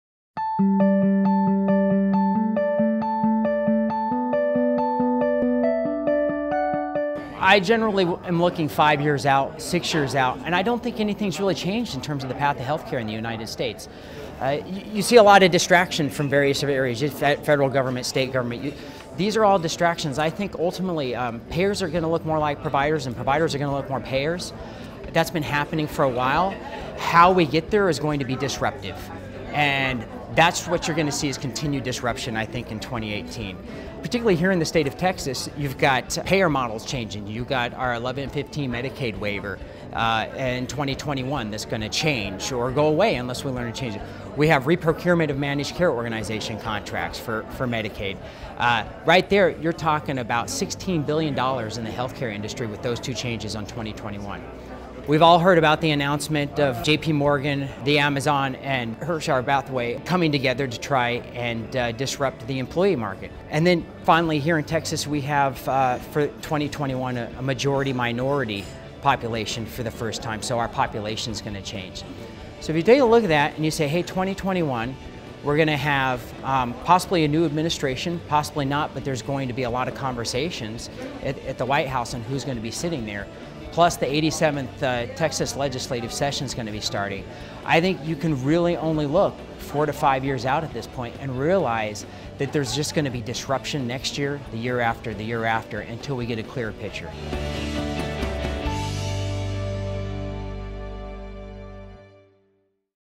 What They're Watching: Kevin Denmark
Kevin Denmark is the Vice President of Client Partnerships at Beacon Health Options. He joins us in this edition of "What They're Watching" To talk about the next five years in healthcare.